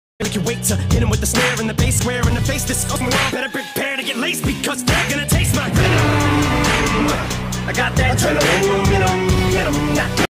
Time For More Paleoart!!
The original image is of a left-lateral view of the skull and neck vertebrae of Stan the Tyrannosaurus (I think).